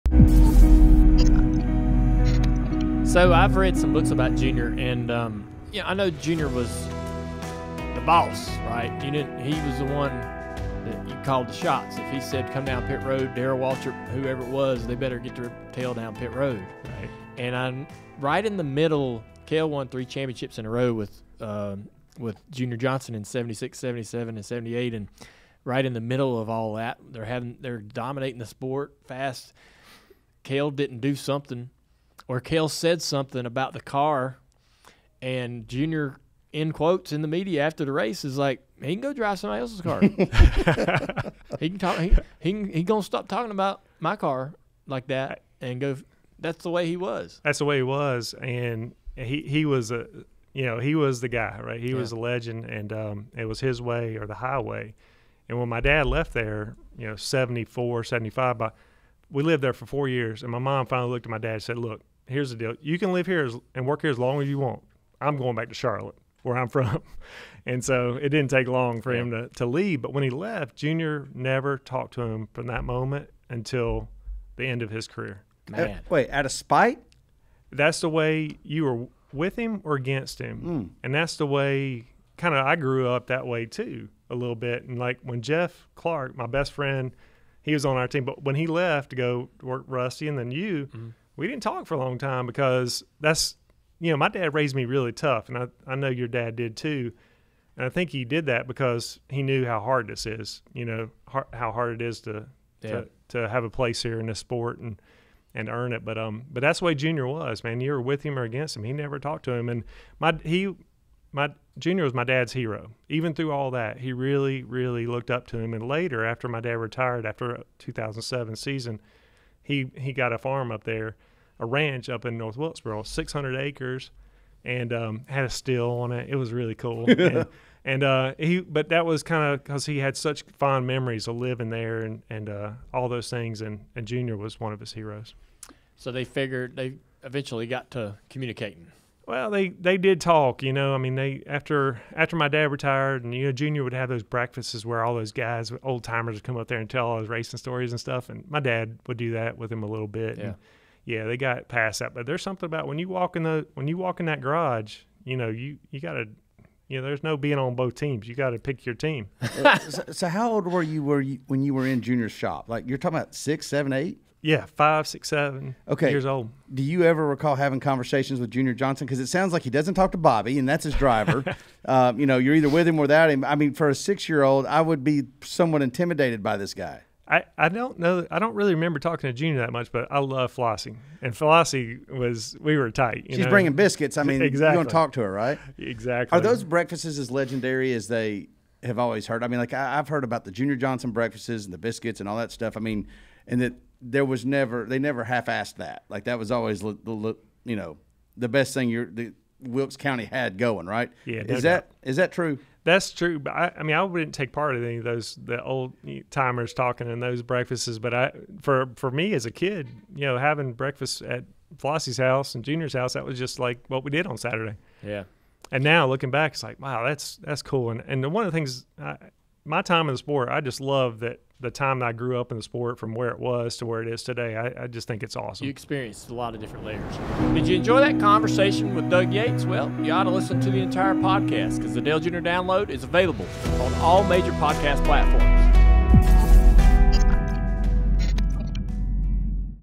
Doug Yates Recalls Childhood With Junior Johnson
Growing up in North Carolina, the epicenter of modern stock car racing, Doug Yates knew nothing different than his father Robert working on engines. But life in Charlotte shifted to the hills of North Wilkesboro. The Yates family uprooted, and went to work for NASCAR car-owner and folk hero Junior Johnson.